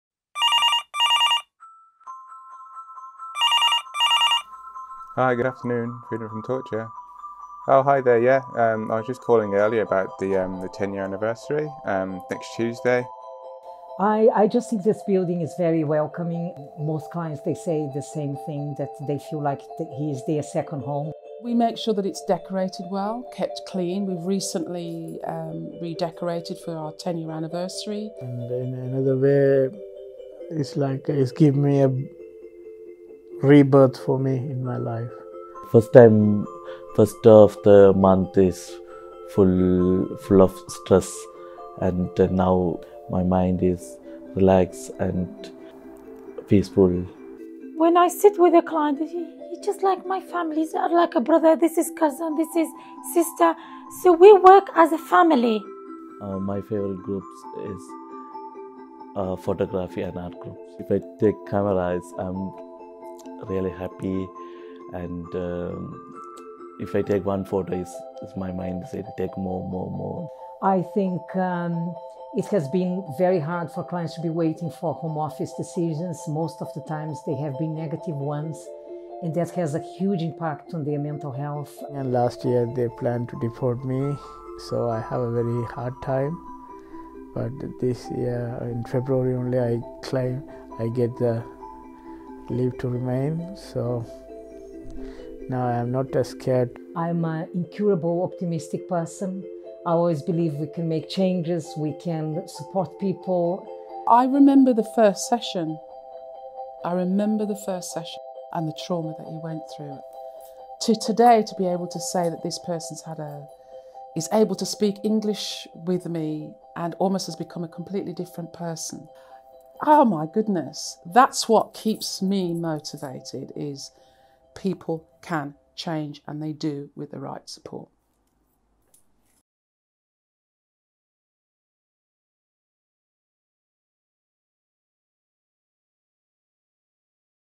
Birmingham Center's 10th Birthday
A celebration of 10 years of our West Midland Centre.

Hear from our clients, therapists and staff about what makes Freedom from Torture special.